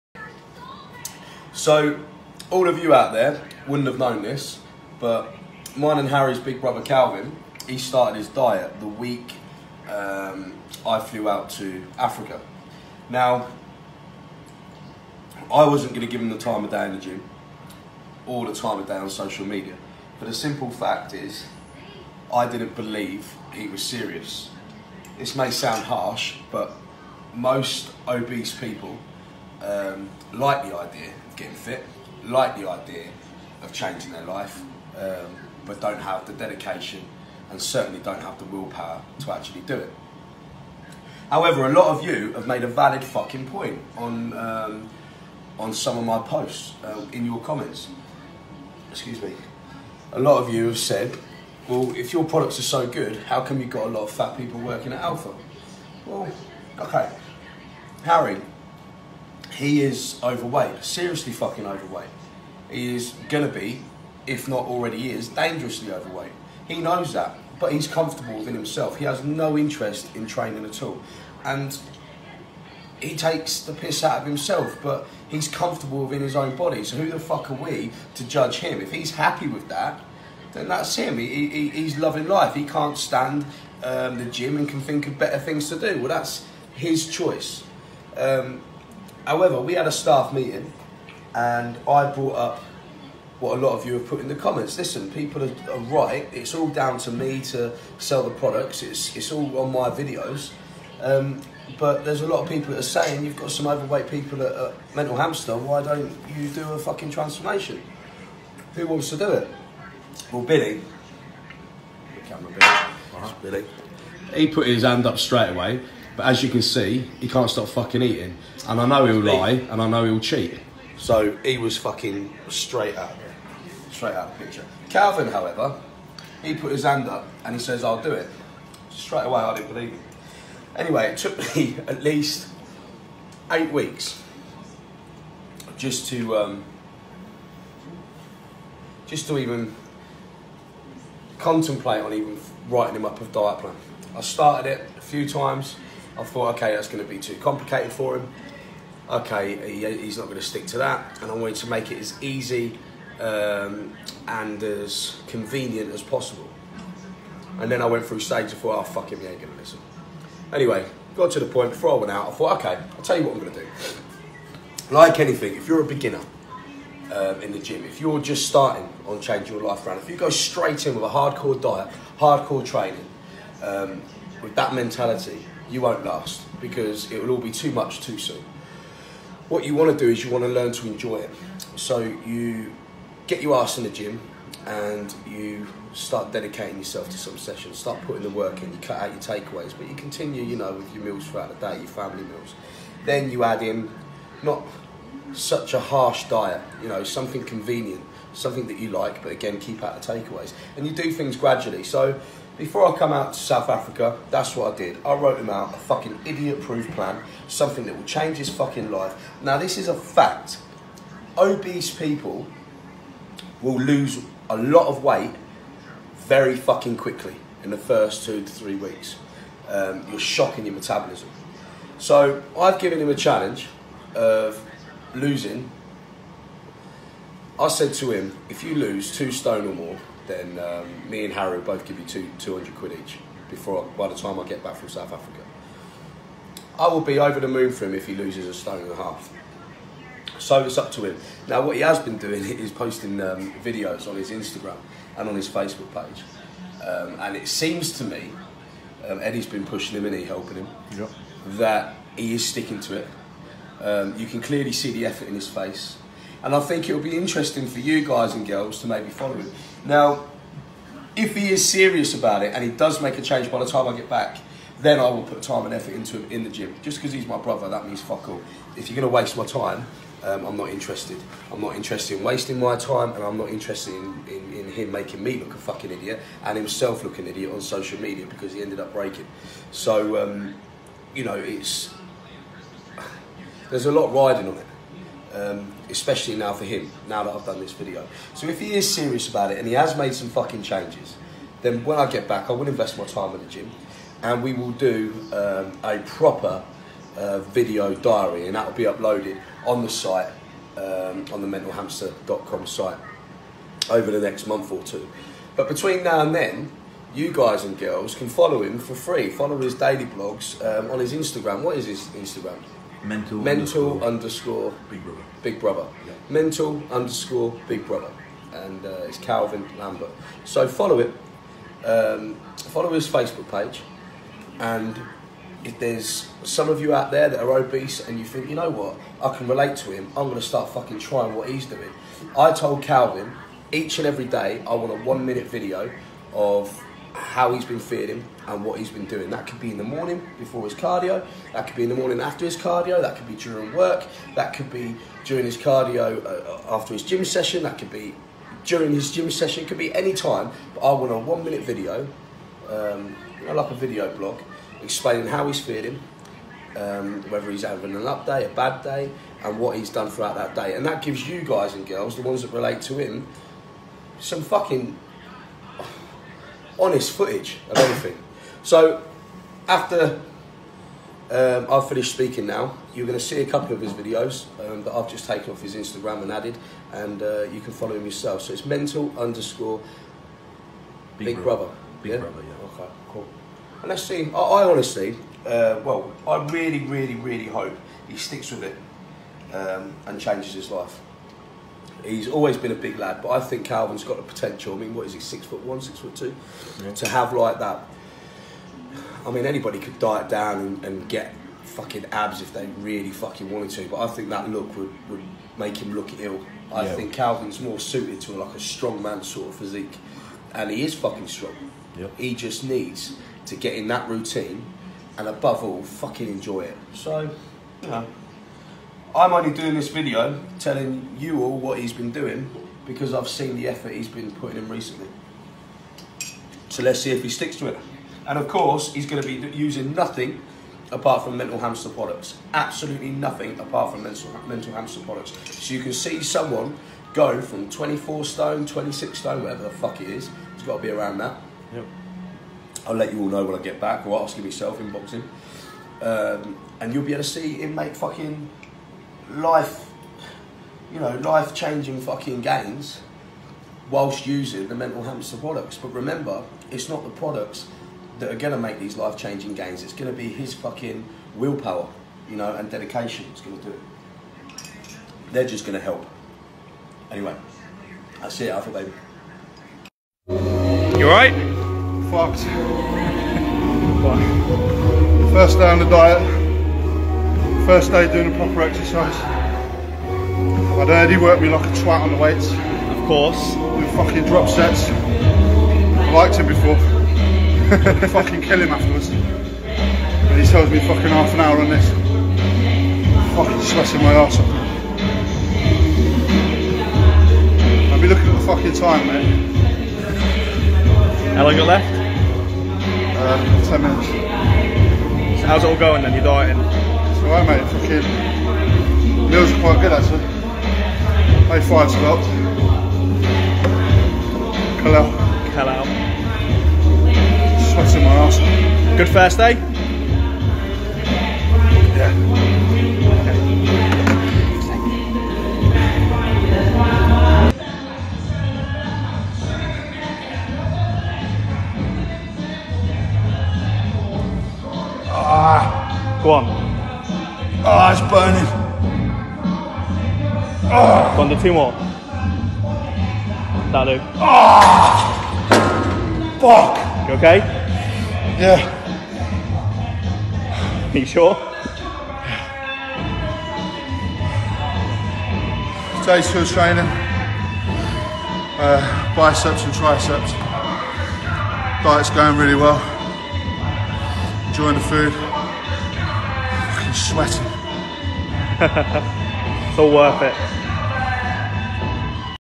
Aarron Lambo - This is as REAL as it gets for transformation using Mental Hamster products!
This is as REAL as it gets for transformation using Mental Hamster products!!

I don’t care if he’s my brother or not the fact is most obese people like the ‘idea’ of changing their life but don’t have the will power or dedication to do it..

However working off a beginner plan I made up specifically for Calvin and his life style this is his chance to prove to me while I’m away that he has what it takes. If he sticks with it and makes some noticeable changes over the next two weeks then and only then I’ll put the effort in when I’m back and really step things up!!

Maybe YOU relate to his situation and want to get inspired, if so follow his journey on his Instagram

mental_bigbrother

MentalHamster.com